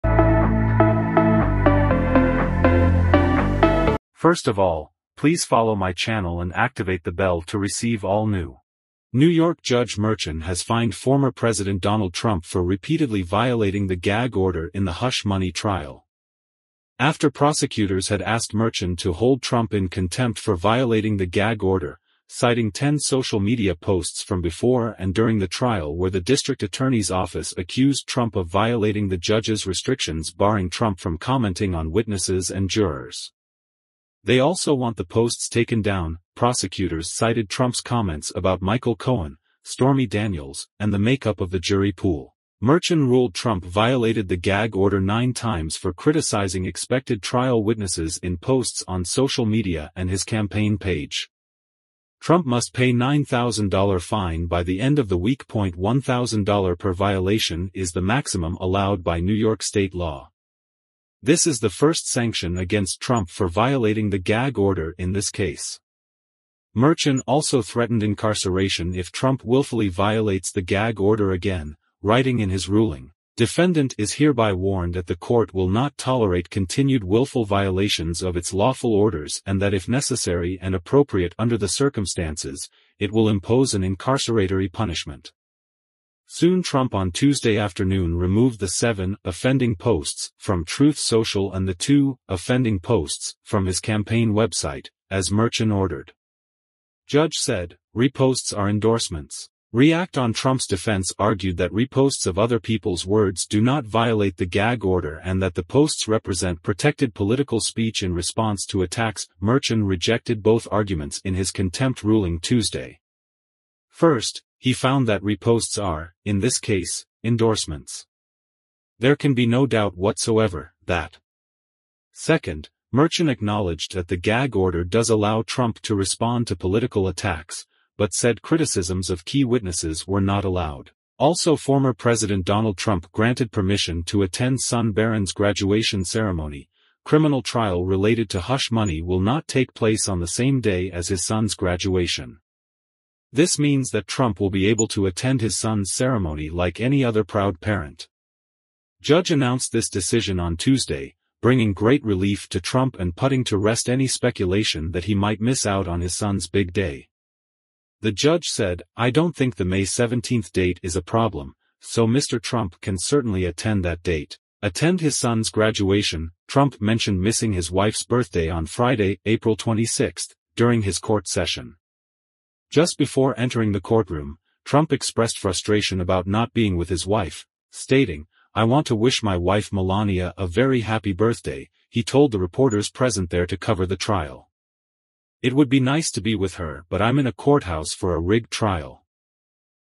Judge fined Trump for violating gag order who granted permission to attend Barron's gradution#trump
New York Judge Merchan has fined former President Donald Trump for repeatedly violating the gag order in the hush money trial. after Prosecutors had asked Merchan to hold Trump in contempt for violating the gag order, citing 10 social media posts from before and during the trial where the district attorney’s office accused Trump of violating the judge’s restrictions barring Trump from commenting on witnesses and jurors. They also want the posts taken down.Prosecutors cited Trump’s comments about Michael Cohen, Stormy Daniels and the makeup of the jury pool.
Merchan ruled Trump violated the gag order nine times for criticizing expected trial witnesses in posts on social media and his campaign page. Trump must pay nine thousand $  fine by the end of the week.one thousand $  per violation is the maximum allowed by New York State law.
This is the first sanction against Trump for violating the gag order in this case.
Merchan also threatened incarceration if Trump willfully violates the gag order again, writing in his ruling, Defendant is hereby warned that the Court will not tolerate continued willful violations of its lawful orders and that if necessary and appropriate under the circumstances, it will impose an incarceratory punishment.
soon Trump on Tuesday afternoon removed the seven “offending posts” from Truth Social and the two “offending posts” from his campaign website, as Merchan ordered.
 judge said, Reposts are endorsements, react on Trump’s defense argued that reposts of other people’s words do not violate the gag order and that the posts represent protected political speech in response to attacks.Merchan rejected both arguments in his contempt ruling Tuesday.
First, he found that reposts are, in this case, endorsements.
“There can be no doubt whatsoever, that
Second, Merchan acknowledged that the gag order does allow Trump to respond to political attacks, but said criticisms of key witnesses were not allowed.
 also Former President Donald Trump granted permission to attend son Barron's graduation ceremony,  criminal trial related to hush money will not take place on the same day as his son's graduation. This means that Trump will be able to attend his son's ceremony like any other proud parent. Judge announced this decision on Tuesday, bringing great relief to trump and putting to rest any speculation that he might miss out on his son's big day. the judge said"I don’t think the May 17 date is a problem, so Mr. Trump can certainly attend that date, attend his son’s graduation.Trump  mentioned missing his wife's birthday on Friday, April 26, during his court session. Just before entering the courtroom, Trump expressed frustration about not being with his wife, stating, "I want to wish my wife Melania a very happy birthday," he told the reporters present there to cover the trial. It would be nice to be with her but I'm in a courthouse for a rigged trial.",